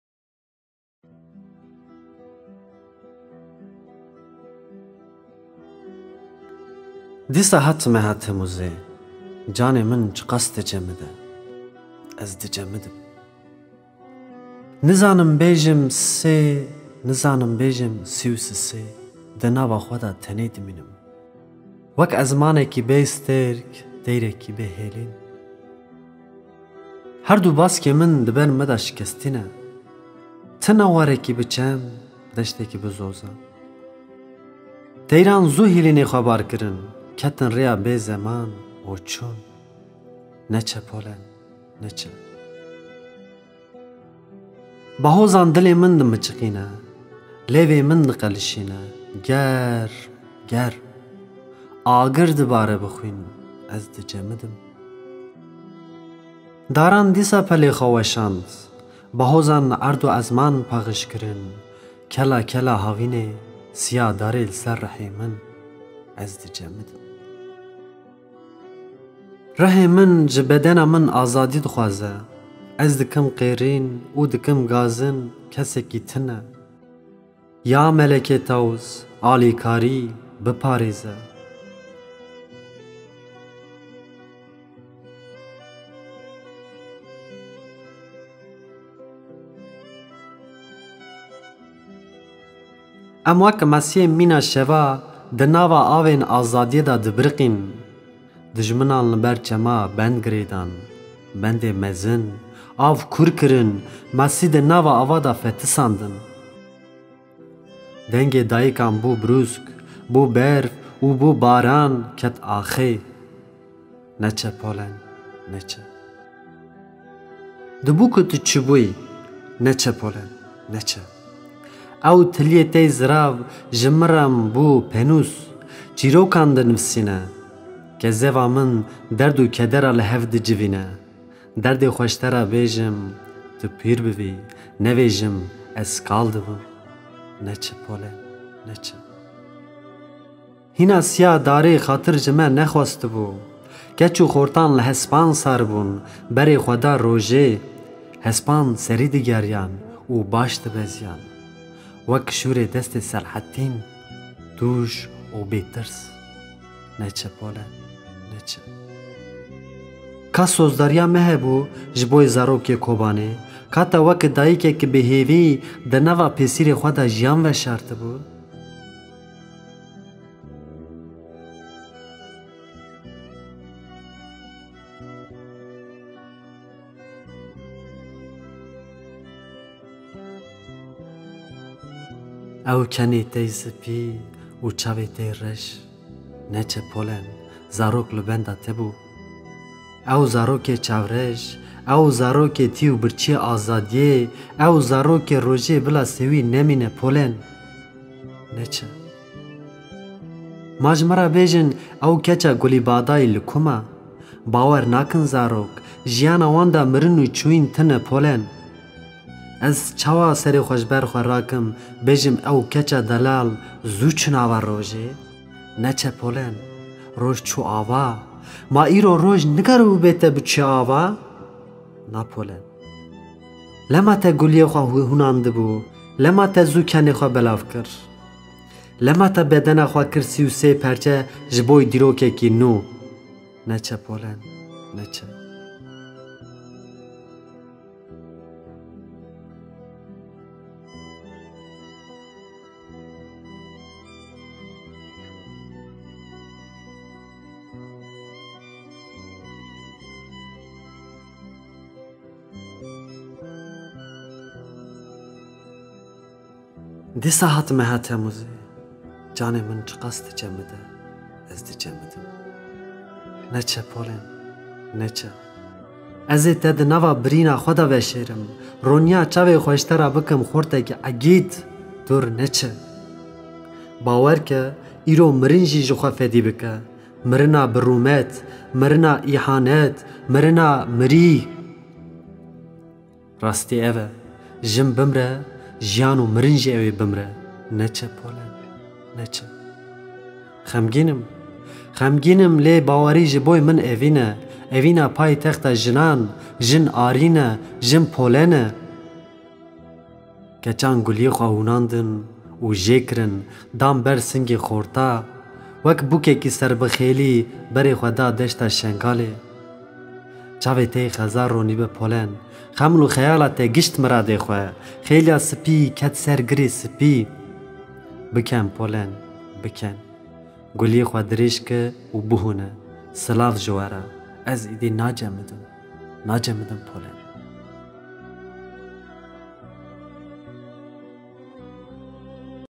Helbest/Şiir Garip çelik
Suruç'ta Şehit düşen 33 yoldaşın anısına: GİTME POLEN

 ji Bo Bîranîna Sî û Sê Şehîdên Pîrsusê : NEÇE POLEN